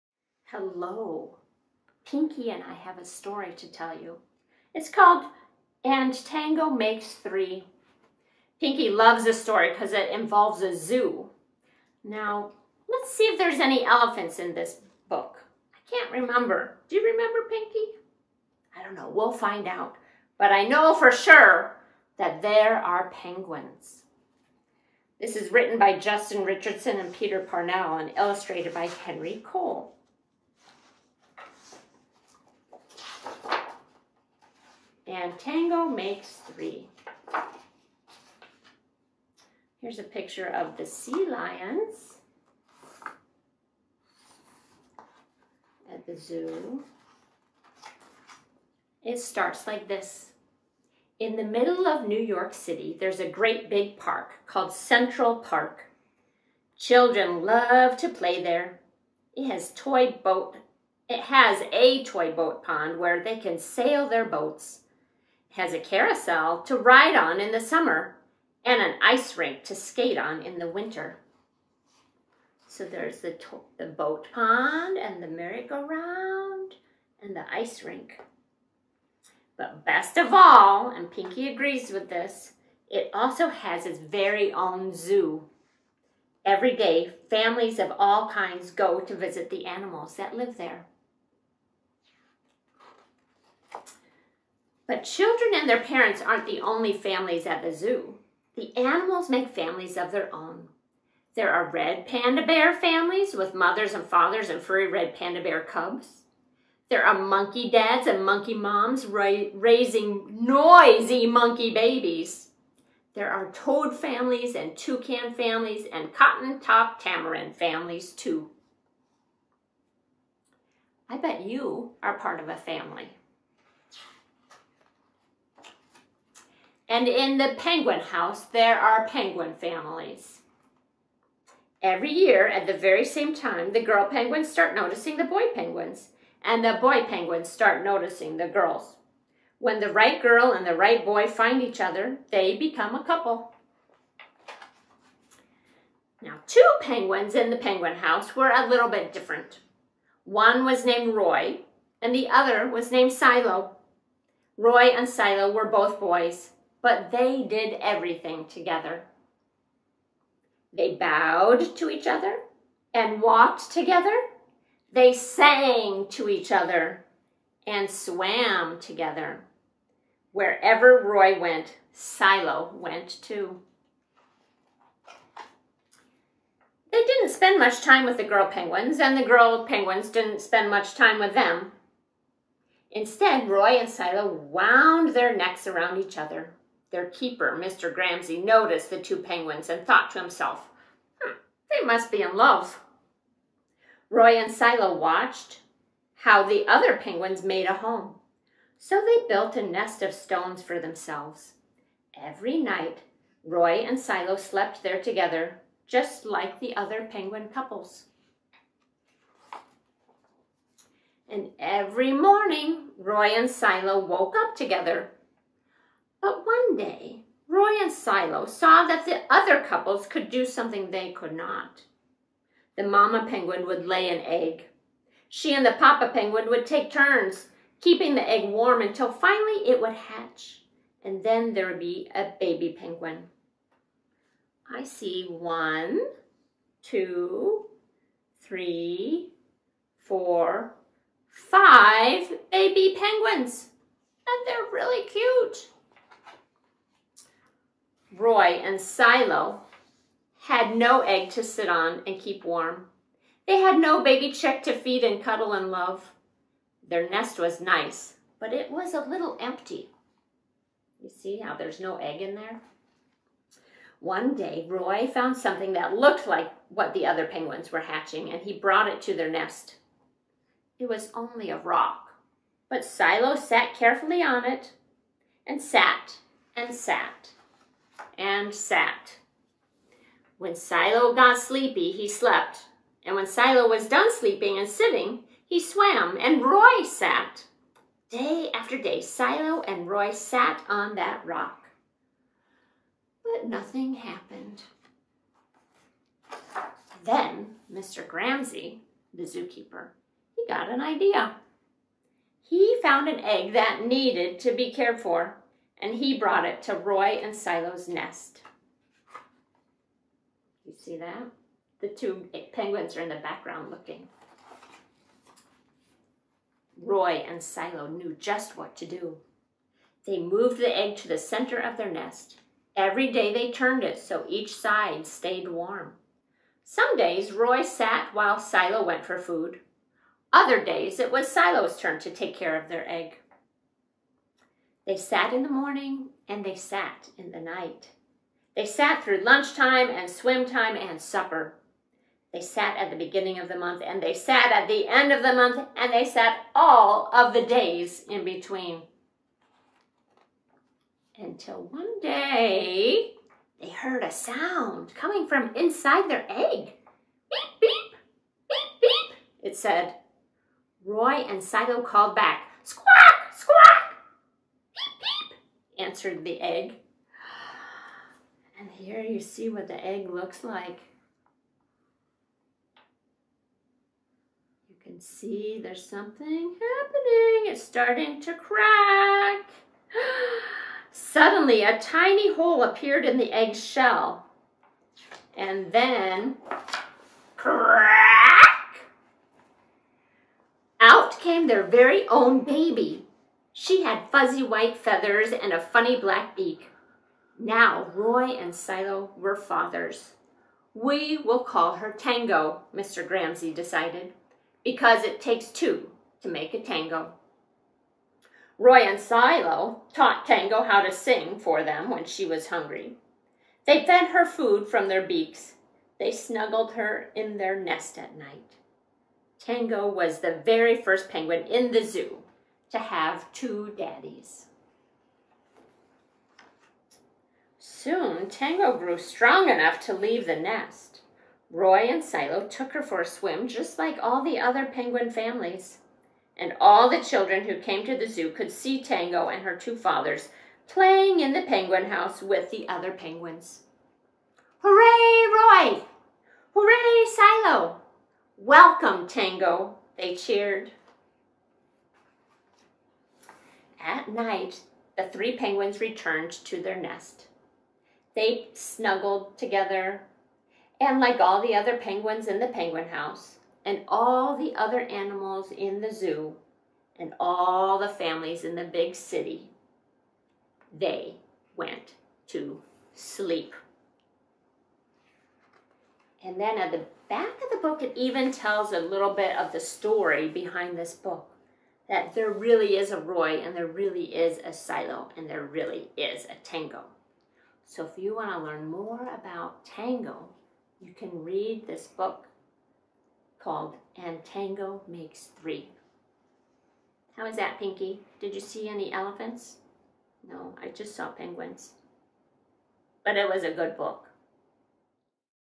Story Time: "And Tango makes Three."
Intern Minister Carin Bringelson and Pinky, read "And Tango makes Three." by Peter Parnell and Justin Richardson.

Filmed and Edited by Daniel Carnes